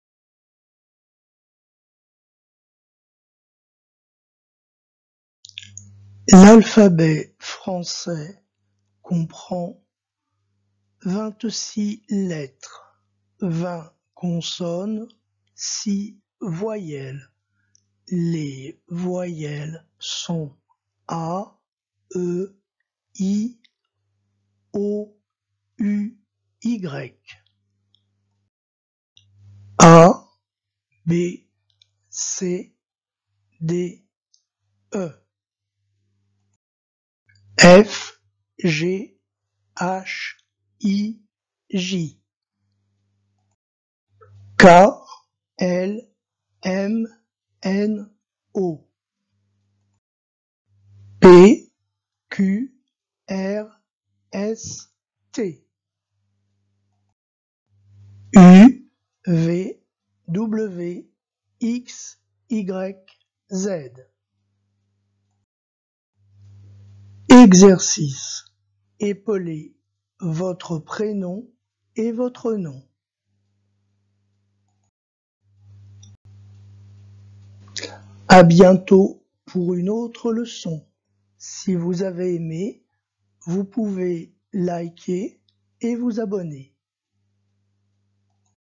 Apprendre l'alphabet français
Dans cette vidéo, je vous apprends l'alphabet français, avec un petit exercice à la fin.